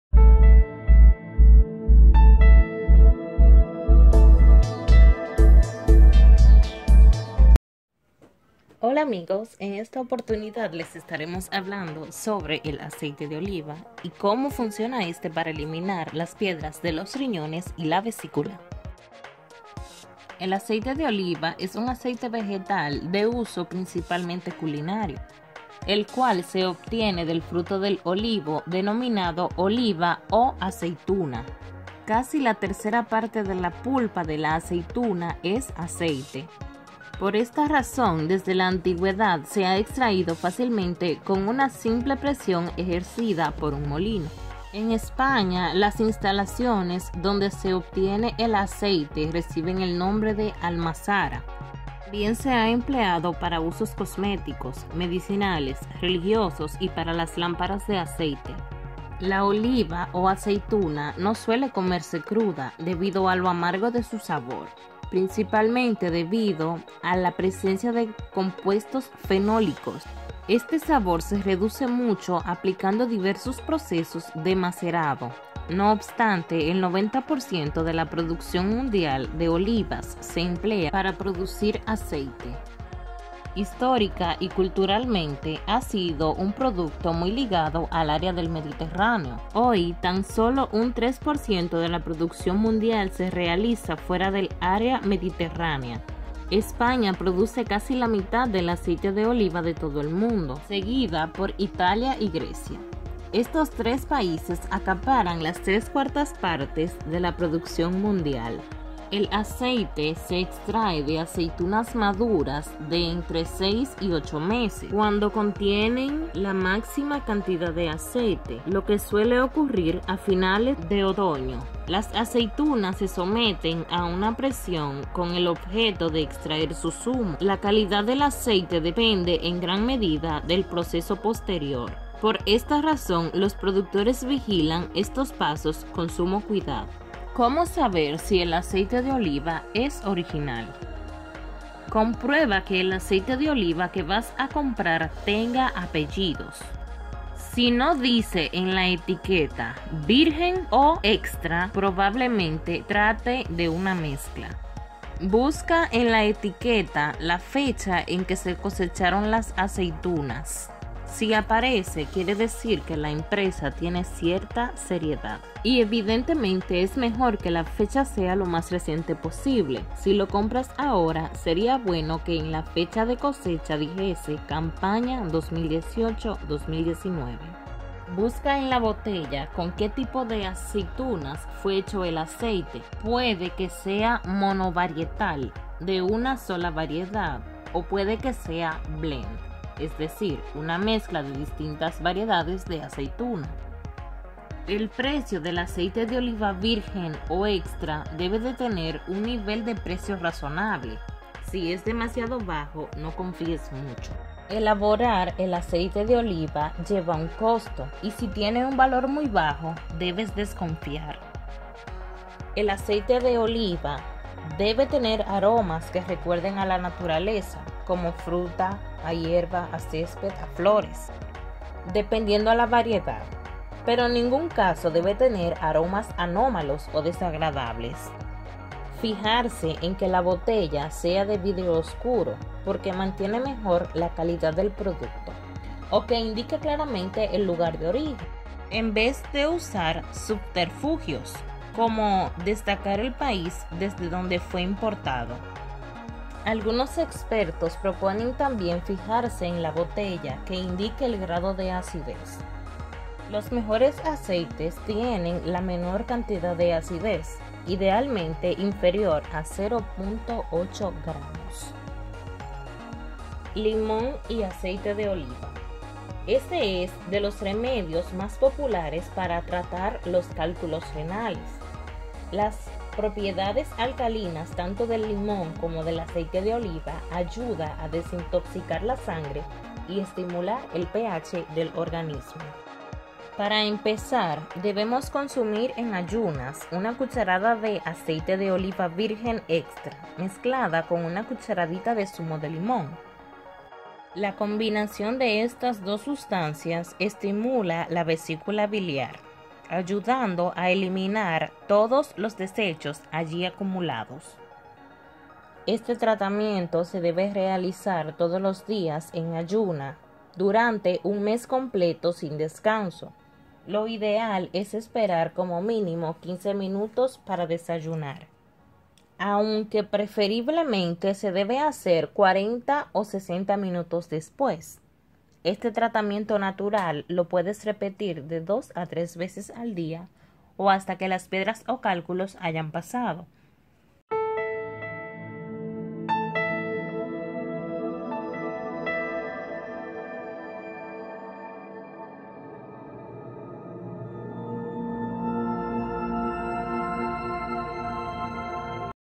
Elimina piedras en riñones y vesícula con Aceite de Oliva y limón. ¿Cómo reconocer un aceite falso?
En este vídeo le enseñaremos la mejor forma y la mas rapida de preparar este remedio tradicional para eliminar los CalculosRenales, así como que debe saber para elegir el mejor aceite de oliva.

Puede usar y realizar este remedio en el envase que guste pero en las proporciones indicadas.

Este remedio es muy eficaz para destruir las PiedrasenlosRiñones, nosotros lo hacemos de esa forma para asegurarnos de hacerlo lo mas fresco posible, y como son dos cucharadas lo hacemos dos veces en la misma cuchara con iguales proporciones.